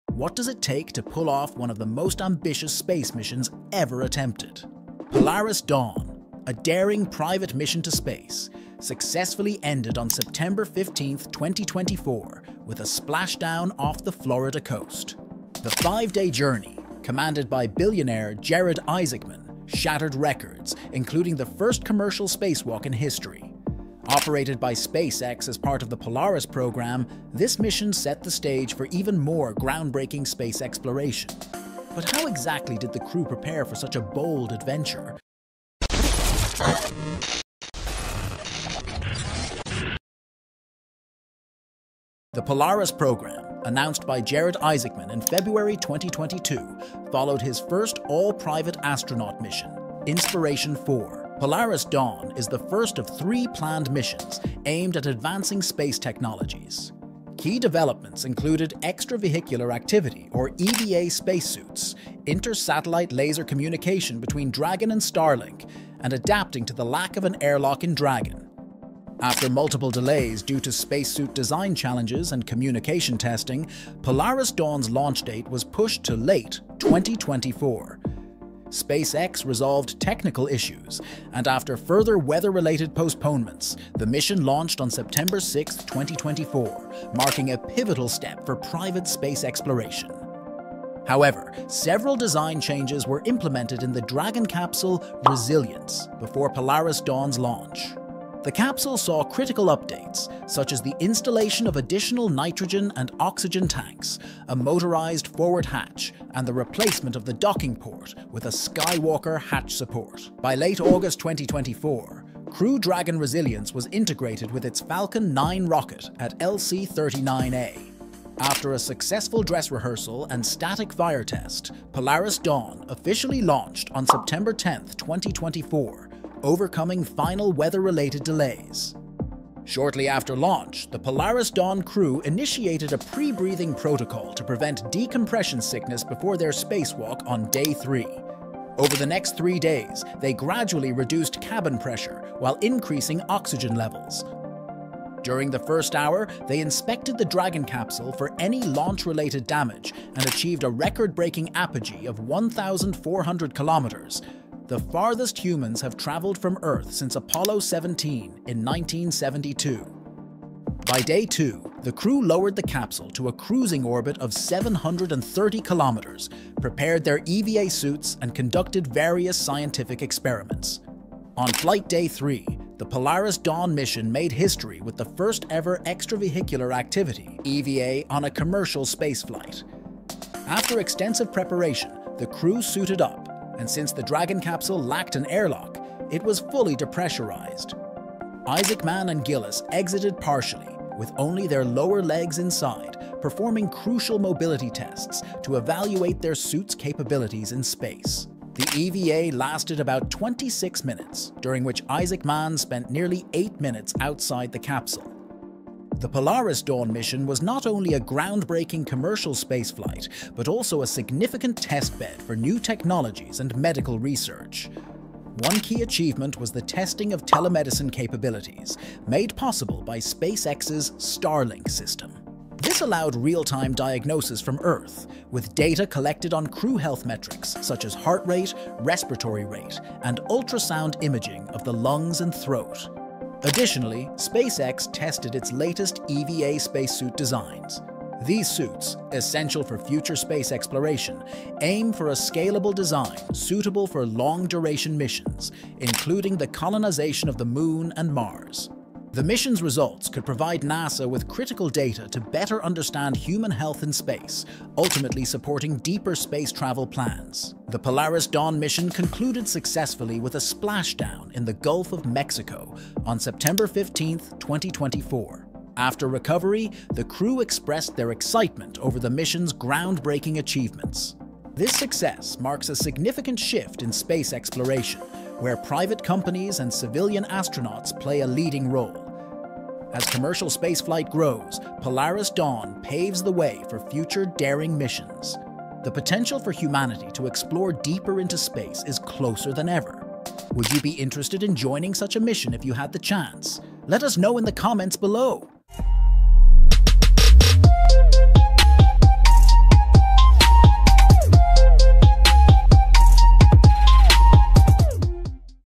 SpaceX's Polaris Dawn: The Space Mission That Will Change Everything!
🚀 Polaris Dawn: SpaceX’s Most Daring Space Mission! 🌌

What makes Polaris Dawn so groundbreaking? 🌍 In this video, SciTech takes you inside SpaceX's most ambitious space mission to date! Get ready to explore the incredible plans for spacewalks, orbital research, and even human spaceflight milestones. This mission is pushing the boundaries of what's possible in space exploration! 🌠

We’ll dive into:

🌍 What Polaris Dawn means for the future of space travel

🧑‍🚀 The first-ever commercial spacewalk and what it entails

🪐 How SpaceX is working towards reaching Mars through missions like this

🛰️ The advanced technologies and goals behind Polaris Dawn

Join us on a thrilling journey into outer space as we break down the details of this epic mission. Buckle up! 🚀💫